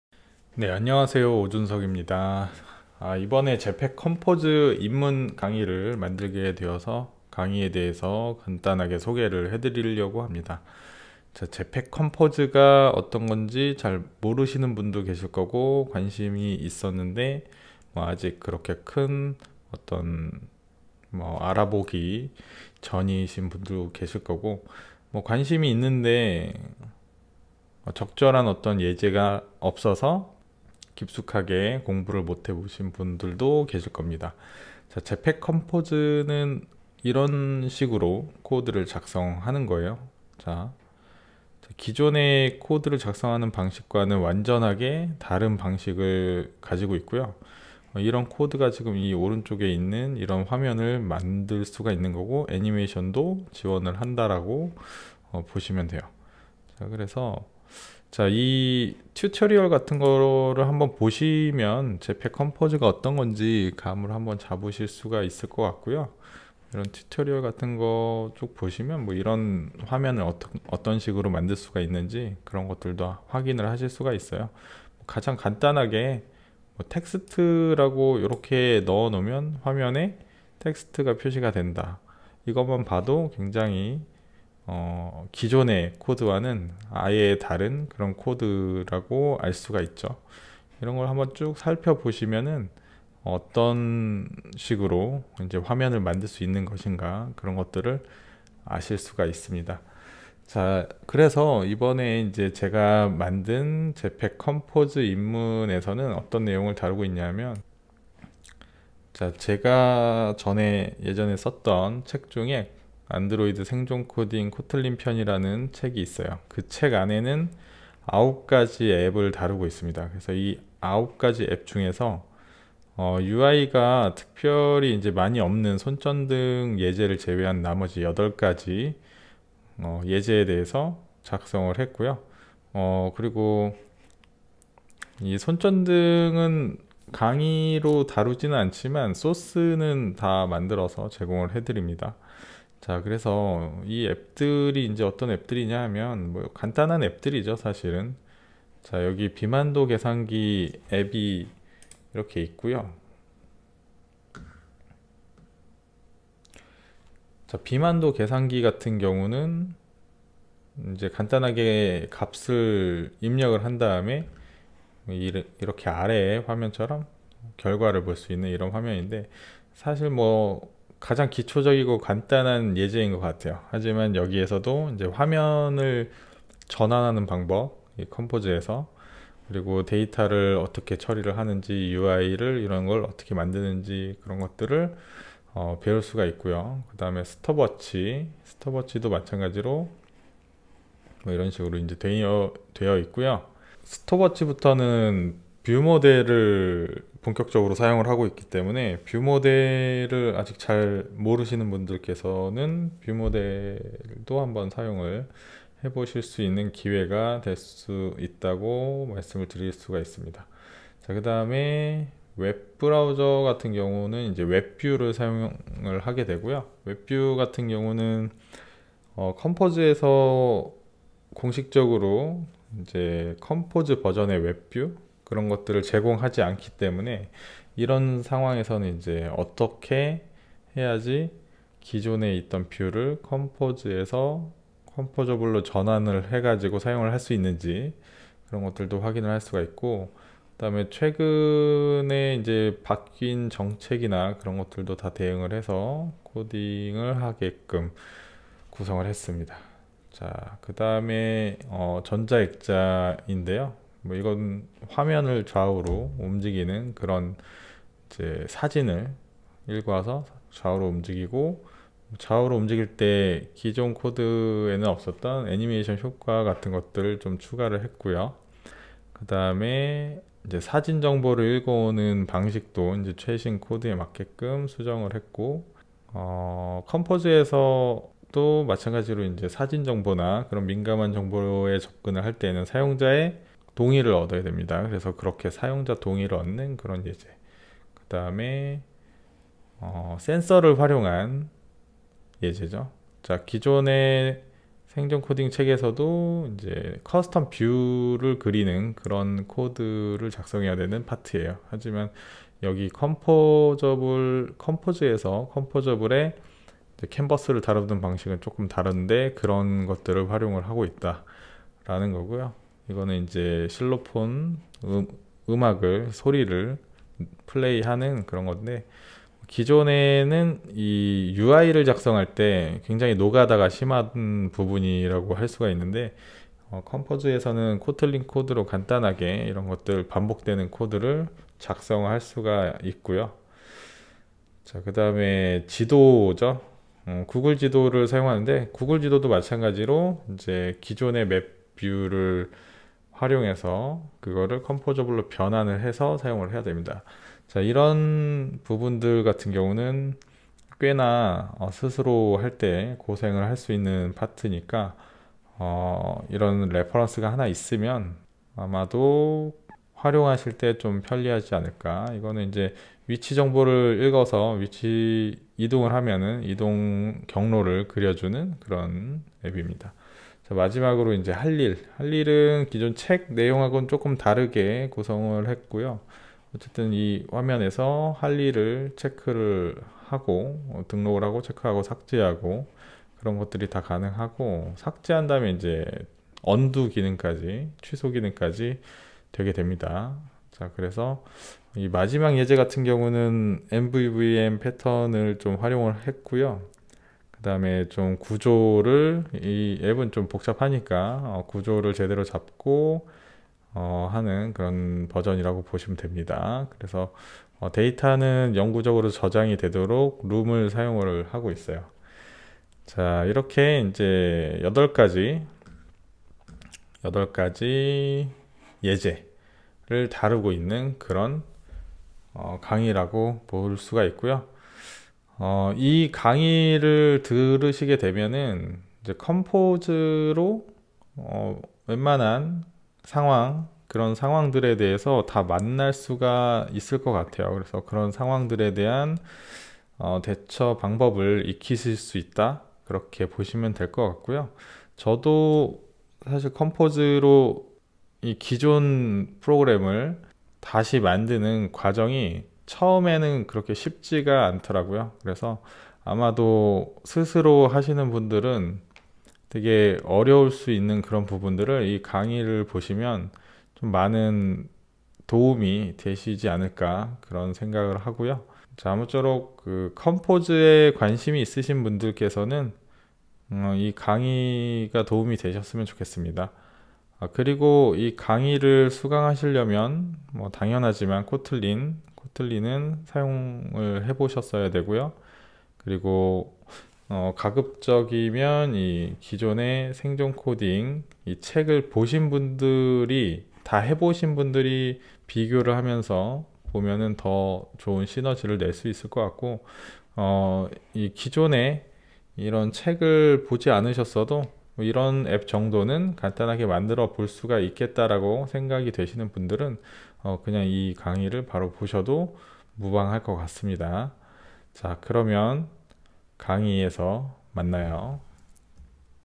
새로운 강의 소개 - 모던 안드로이드 Jetpack Compose 입문
안드로이드의 새로운 UI 개발 툴킷 Jetpack Compose 입문 유료 강의입니다.
유튜브에 공개된 영상을 포함하고 있으며 클론 코딩 8가지가 추가됩니다.